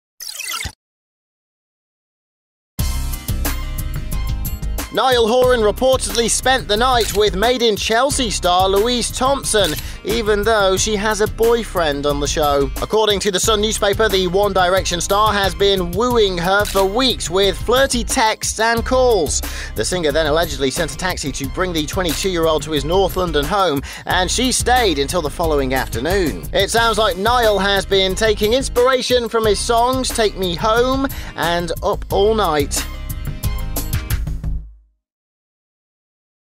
Niall Horan Has a Secret Date With Made In Chelsea's Louise Thompson - Splash News | Splash News TV
Niall Horan reportedly spent the evening with Made In Chelsea star Louise Thompson after wooing her for weeks.